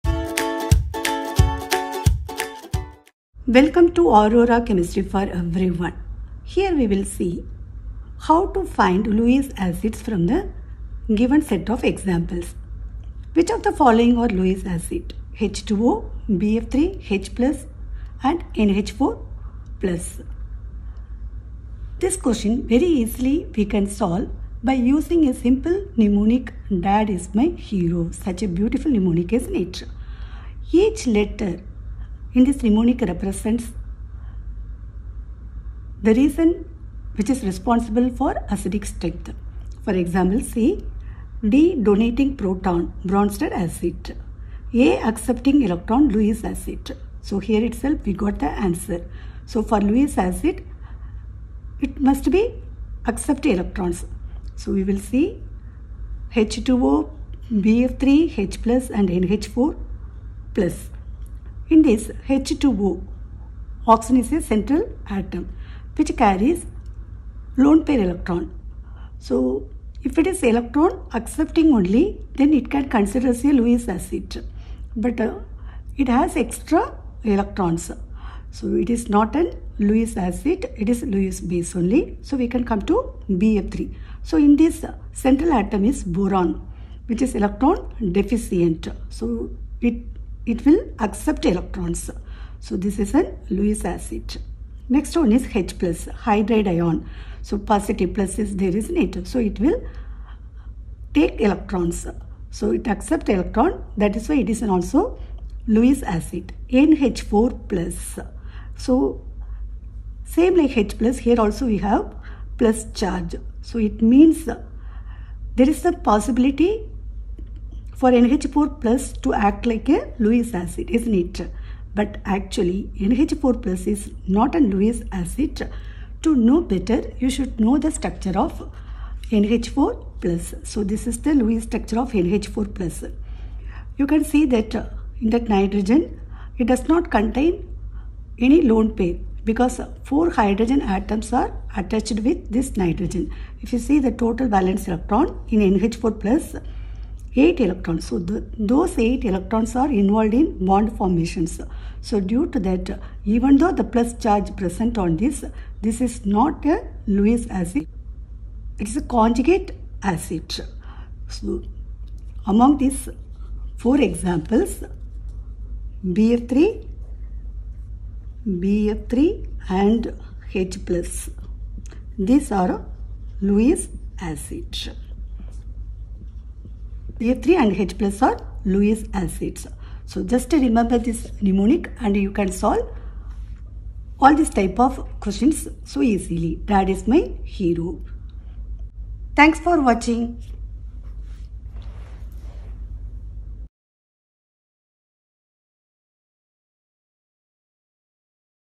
Which of the following are Lewis acid ? H2O, BF3, H+, NH4+
Lewis acids are electron acceptors. Here we will discuss how to identify Lewis acids easily.
Lewis acids are electron donors.Among H2O, BF3, H+, NH4+,
  BF3 and H+ are Lewis acids.
( 1.40 - Hydrogen ion is misspelled as hydride ion)
By using the given mnemonic , we can predict the answer in Just one minute.
               DAD IS MY HERO
 D- Donating proton
A- Accepting electrons
D- Difference in hybrid orbitals

 I  - Inductive effect
S - Stable conjugate base

M - Mega size for Hydracids , Hydrides & Halides
Y - Yield H+ ions & make Ka larger

H-  Higher oxidation state for  Oxides & oxyacids except Phosphorus
E - EWG group
R - Resonating structures
O - Low pH